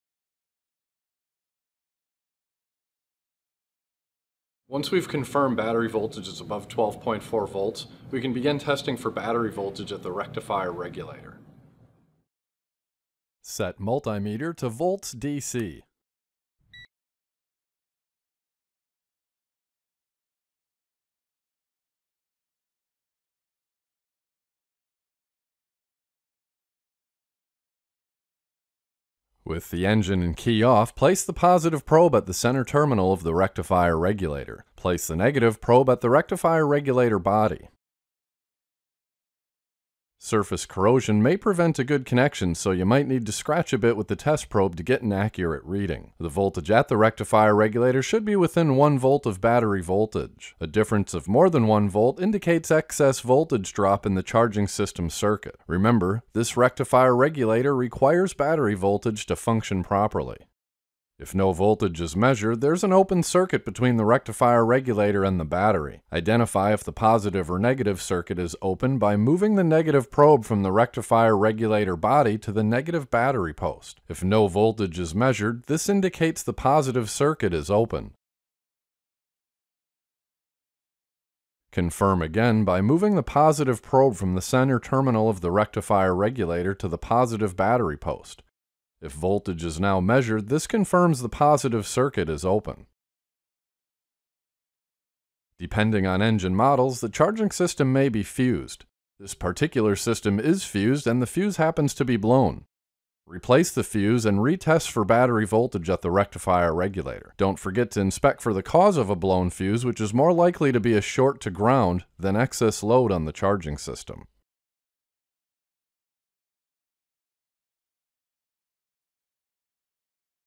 How to Test a KOHLER Voltage Regulator (Rectifier-Regulator)-Battery Voltage Test at Regulator
KOHLER Command PRO EFI 25hp Charging System Diagnosis
Chapter 4: Test Battery Voltage at Rectifier-Regulator
Video 4 of 9
Read Manuals and Clearly Understand ALL Safety Warnings!